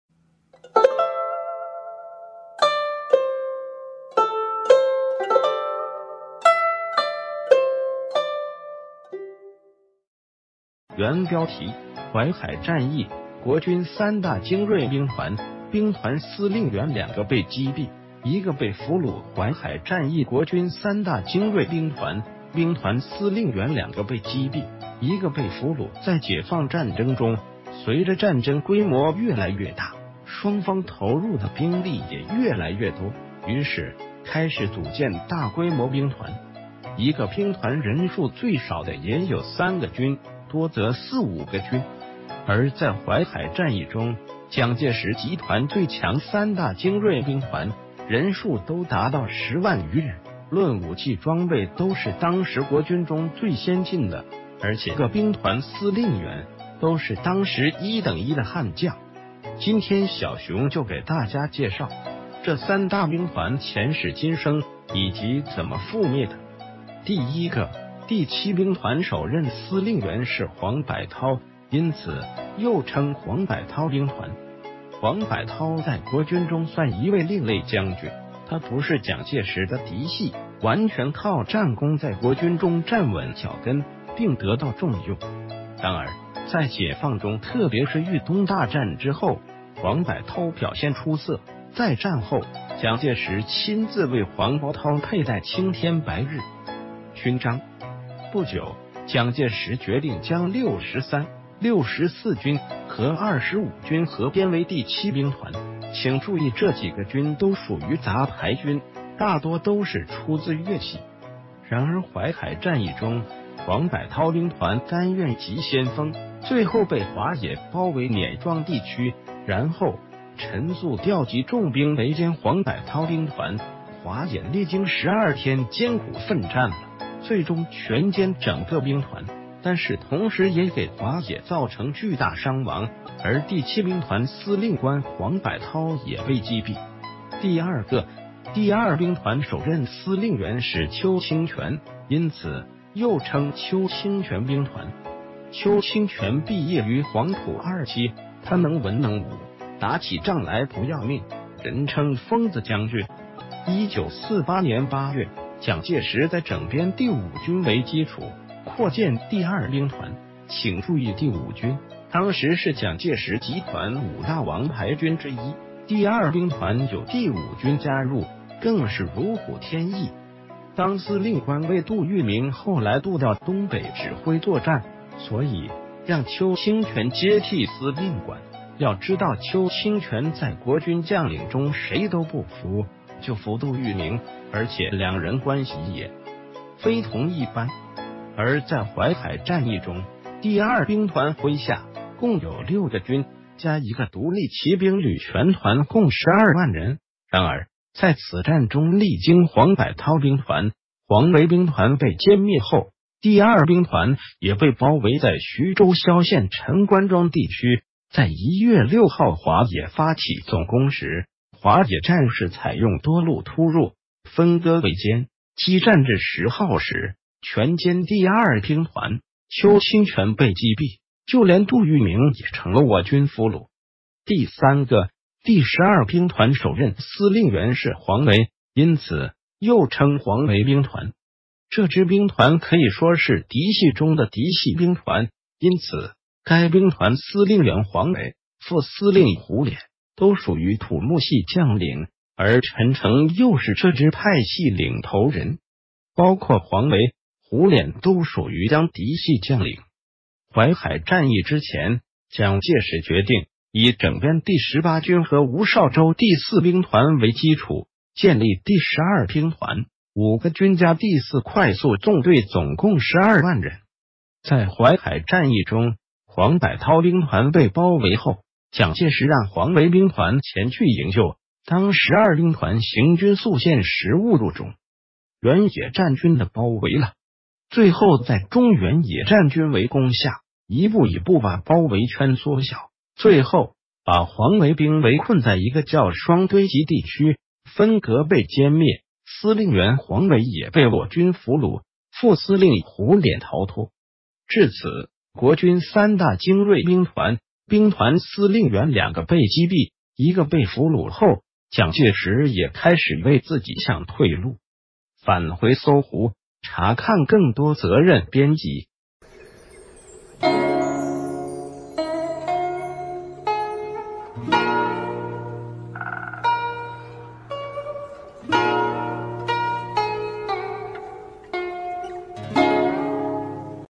淮海戰役：國軍三大精銳兵團，兵團司令員兩個被擊斃，一個被俘虜_蔣介石
原標題：淮海戰役：國...